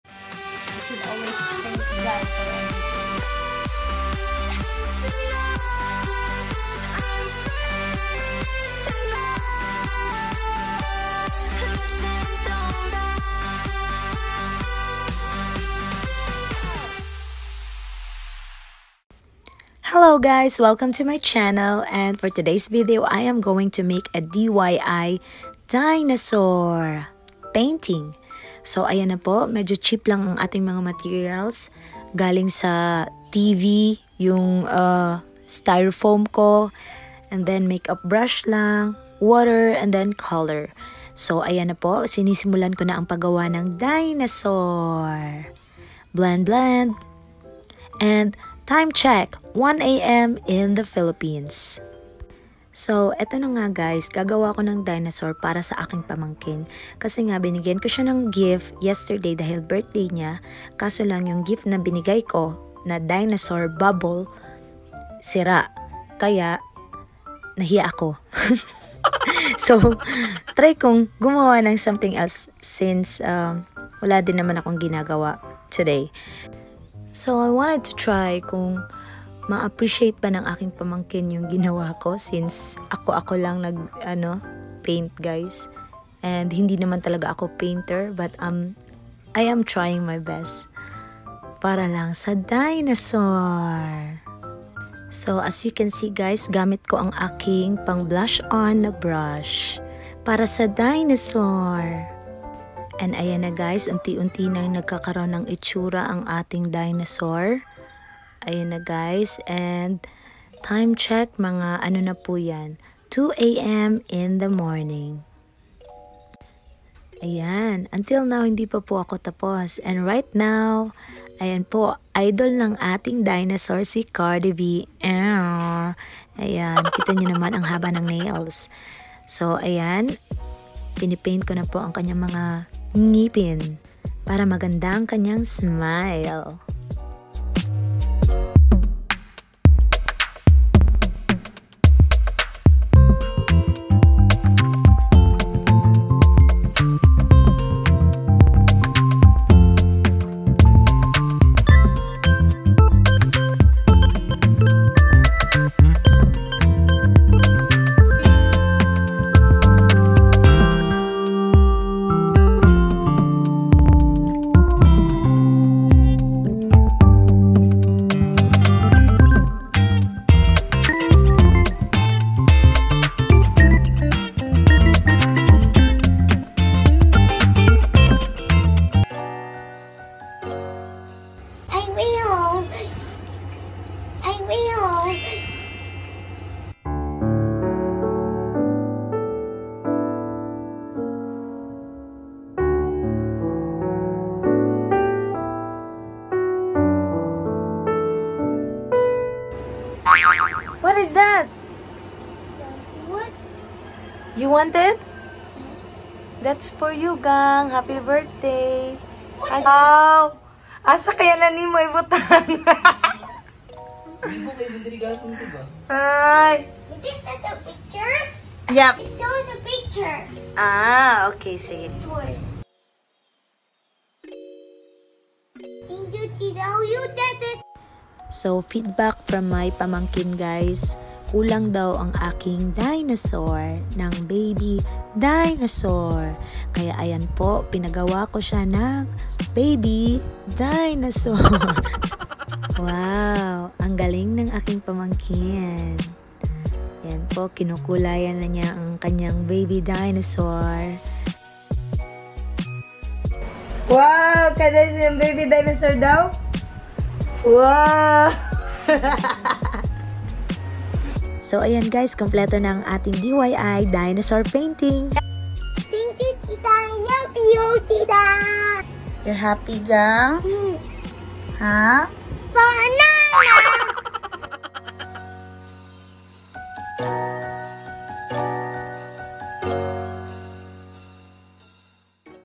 DINOSAUR PAINTING DIY GIFT for my 5 yr old nephew | LARA PATRICIO VLOG
To my Nephew alexzues..I want you to know that I love you gang and you are the sweetest kid evahhh..I'am forever grateful to be your Tita!:)